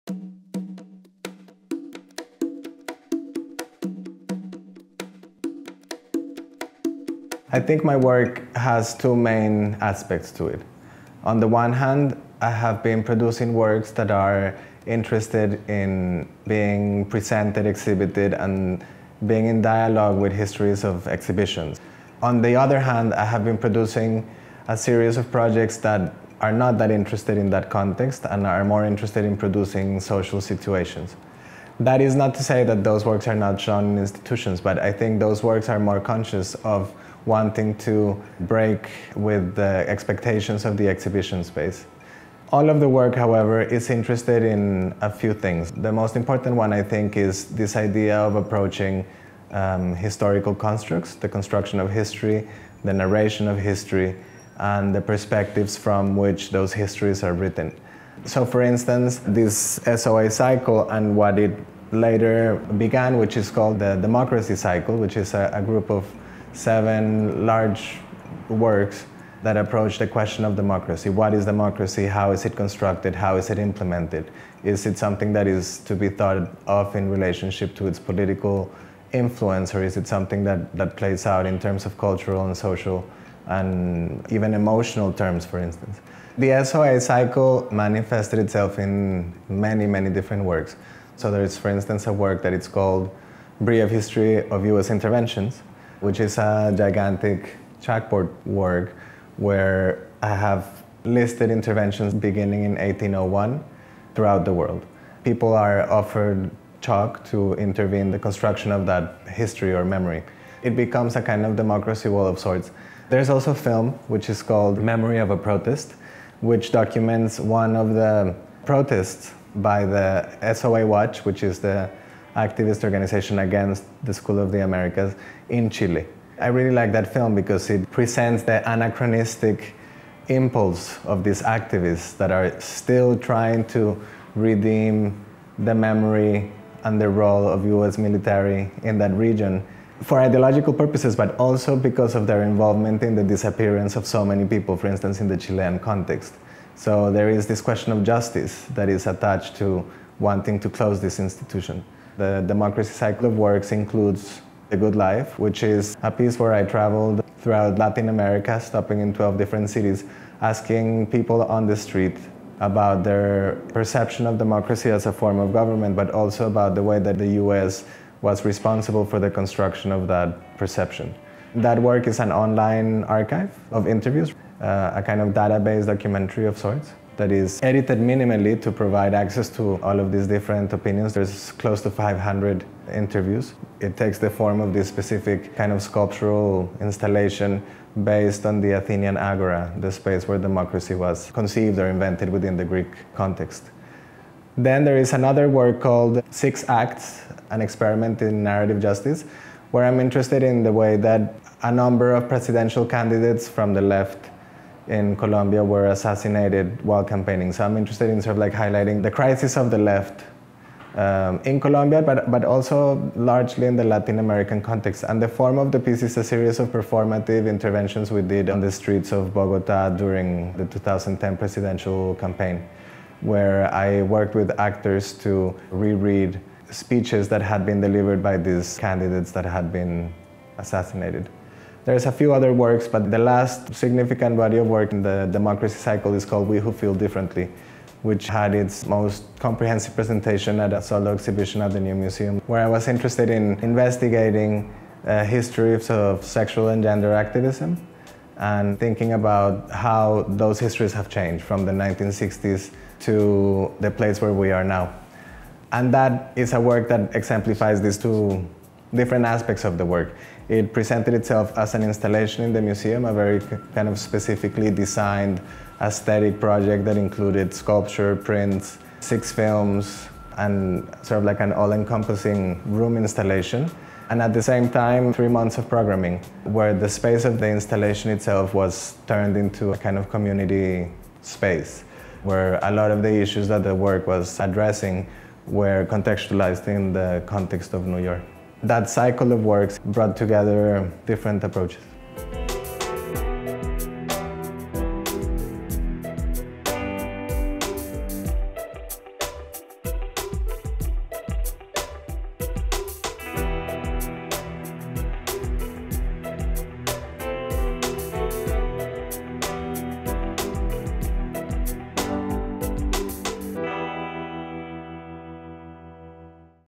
Artist Profile: Carlos Motta on His SOA / Democracy Cycle Works
Learn more about artist Carlos Motta and the Guggenheim UBS MAP Global Art Initiative

Carlos Motta characterizes the two main strands of his work as being concerned with, on the one hand, a conventional exhibition context and, on the other, with instigating and problematizing social situations. He also discusses his overarching interest in critiquing historical narrative, and goes on to describe works from his SOA Cycle / Democracy Cycle series, including Memory of a Protest, The Good Life, Six Acts, and We Who Feel Differently, as well as a previous iteration of his collection artwork.